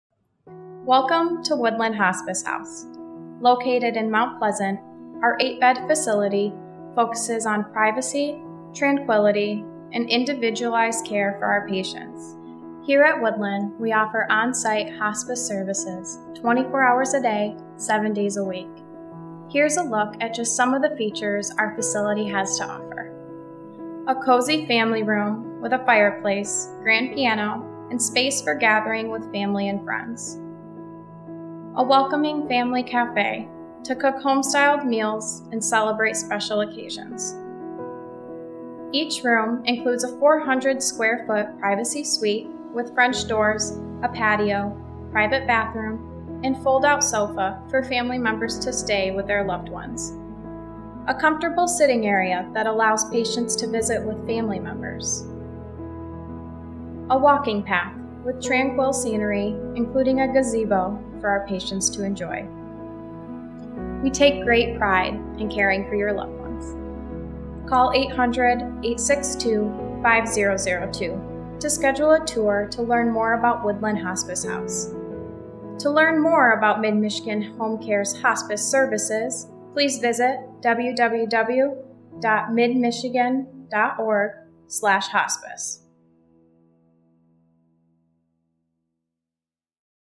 Woodland Hospice House Virtual Tour
MyMichigan Hospice's Woodland Hospice House is a beautiful 8-bed facility focused on privacy, tranquility and individualized care. Its home-like environment provides an alternative living location for short-term respite stays or as a long-term living location. Our on-site staff provides 24 hours per day/7 days per week care.

Services include: A private 400-square foot suite with French doors, a patio, private bathroom and fold out sofa for family members to stay with their loved one; privacy, honoring patient preferences and choices, and achieving maximum comfort; around the clock nursing and hospice aide support to meet individualized needs (able to provide complete care or just minimal supervision and assistance); volunteer, social work and spiritual support; a cozy family room with a fireplace and family café to provide plenty of space for gathering with family and friends. All meal and laundry services are included in room and board.